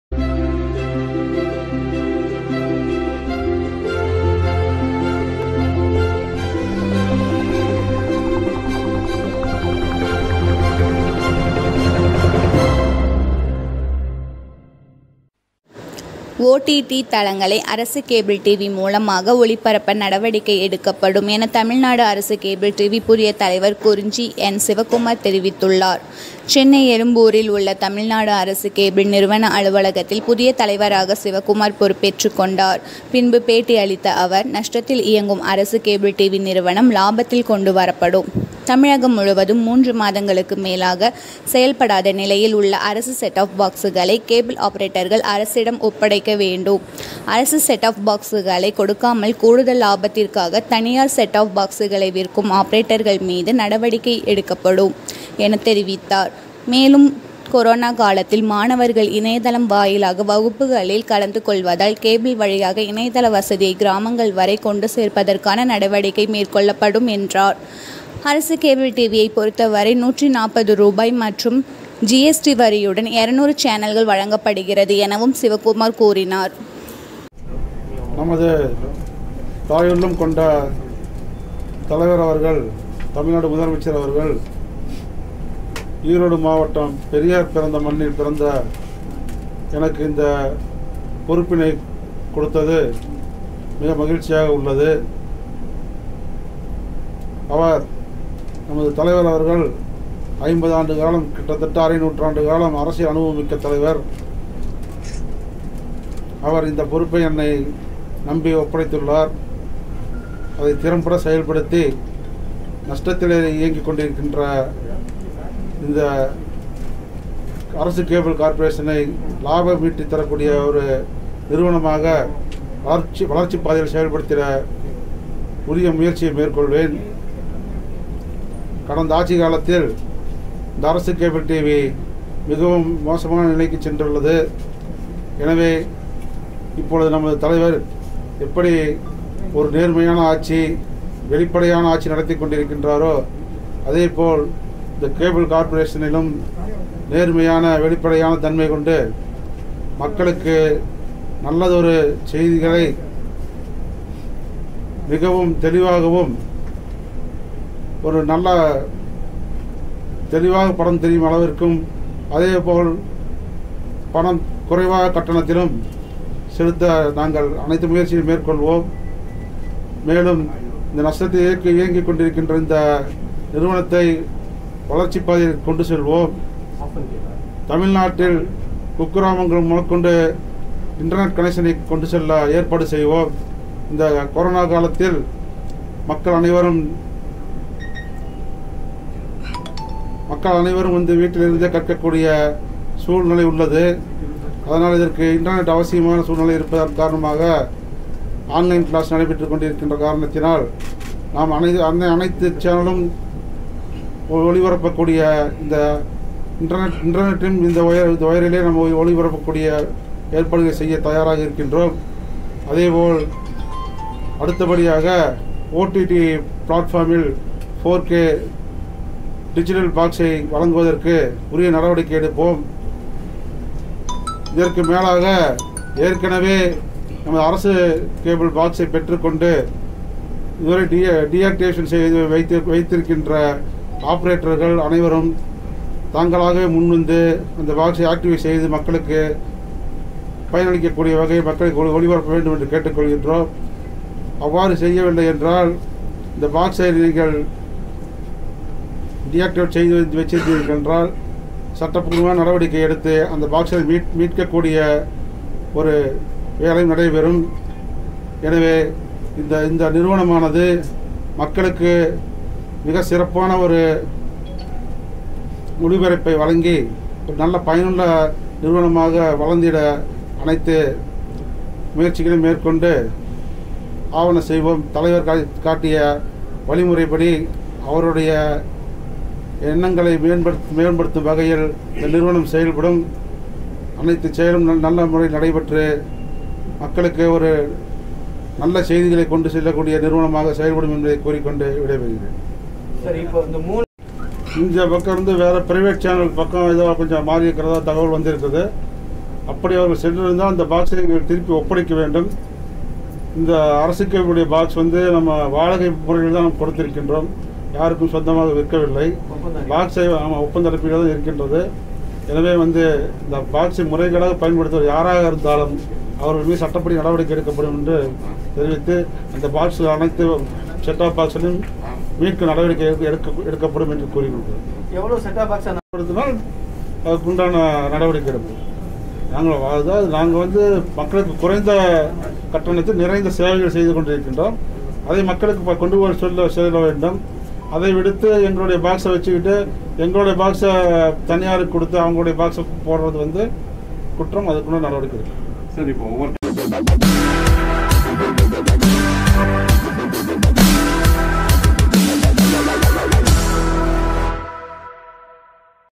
தமிழ்நாடு அரசு கேபிள் நிறுவன அலுவலகத்தில் புதிய தலைவராக சிவகுமார் பொறுப்பேற்றுக் கொண்டார்.
DT NEWS#தமிழ்நாடுஅரசு கேபிள் டி வி OTT தளங்களை அரசு கேபிள் டி.வி மூலமாக ஒளிபரப்ப நடவடிக்கை எடுக்கப்படும் என தமிழ்நாடு அரசு கேபிள் டிவி புதிய தலைவர் குறிஞ்சி என் சிவக்குமார் தெரிவித்துள்ளார்.

சென்னை எழும்பூரில் உள்ள தமிழ்நாடு அரசு கேபிள் நிறுவன அலுவலகத்தில் புதிய தலைவராக  சிவகுமார்
 பொறுப்பேற்றுக் கொண்டார்.

பின்பு பேட்டி அளித்த அவர்

நஷ்டத்தில் இயங்கும் அரசு கேபிள் டி.வி நிறுவனம் லாபத்தில் கொண்டு வரப்படும்.

தமிழகம் முழுவதும் மூன்று மாதங்களுக்கு மேலாக செயல்படாத நிலையில் உள்ள அரசு செட்டாப் பாக்ஸ்களை கேபிள் ஆபரேட்டர்கள் அரசிடம் ஒப்படைக்க வேண்டும். அரசு செட்டாப் பாக்ஸ்களை கொடுக்காமல் கூடுதல் லாபத்திற்காக தனியார் செட்டாப் பாக்ஸ்களை  விற்கும் ஆபரேட்டர்கள் மீது நடவடிக்கை வைக்கப்படும் என தெரிவித்தார். மேலும்

கொரோனா காலத்தில் மாணவர்கள் இணையதளம் வாயிலாக வகுப்புகளில் கலந்து கொள்வதால் கேபிள் வழியாக இணையதள வசதியை கிராமங்கள் வரை கொண்டு சேர்ப்பதற்கான நடவடிக்கை மேற்கொள்ளப்படும் என்றார். அரசு கேபிள் டிவியை பொறுத்தவரை 140 ரூபாய் மற்றும் ஜிஎஸ்டி வரியுடன் 200 சேனல்கள் வழங்கப்படுகிறது எனவும் சிவக்குமார் கூறினார்.